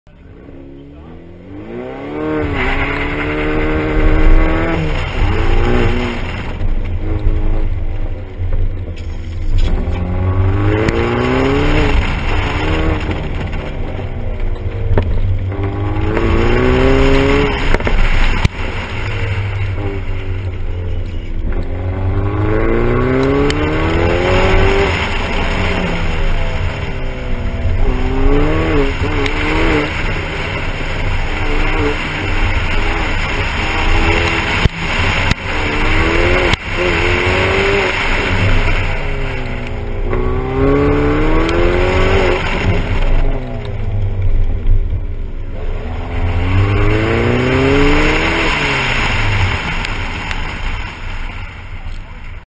SBR 2 2014 Rich Run1
South Bend Region SCCA Autocross Event #2, 2014 @ Tire Rack, South Bend, IN.

ASP 10 - Pontiac Solstice GXP w/ 315 Hoosier A6